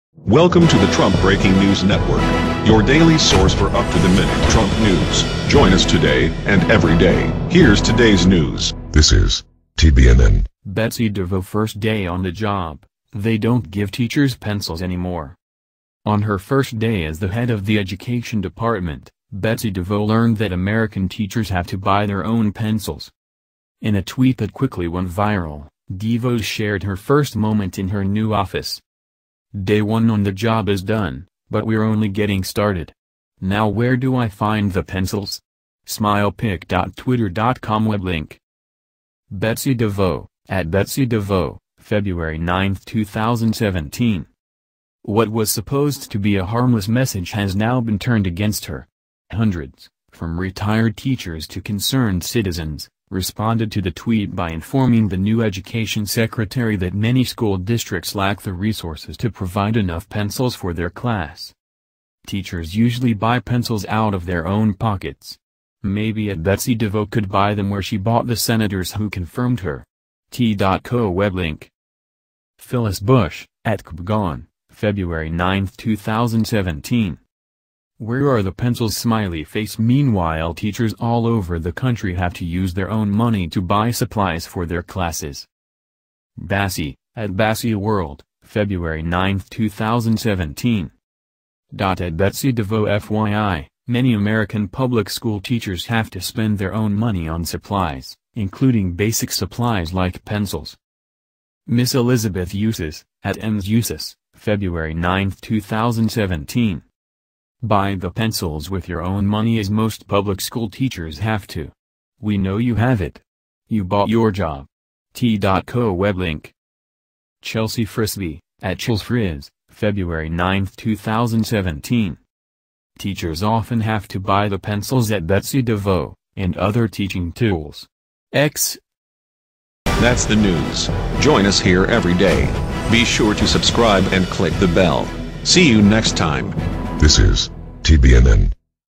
Betsy DeVos’ first day on the job They don’t give teachers pencils anymore
Trump Breaking News Network -Betsy DeVos’ first day on the job They don’t give teachers pencils anymore.

On her first day as the head of the education department, Betsy DeVos learned that American teachers have to buy their own pencils.

In a tweet that quickly went viral, Devos shared her first moment in her new office.

Day 1 on the job is done, but we’re only getting started. Now where do I find the pencils?

— Betsy DeVos (@BetsyDeVos) February 9, 2017

What was supposed to be a harmless message has now been turned against her. Hundreds — from retired teachers to concerned citizens — responded to the tweet by informing the new education secretary that many school districts lack the resources to provide enough pencils for their class.

Teachers usually buy pencils out of their own pockets. Maybe @BetsyDeVos could buy them where she bought the senators who confirmed her.

Category
News & Politics
License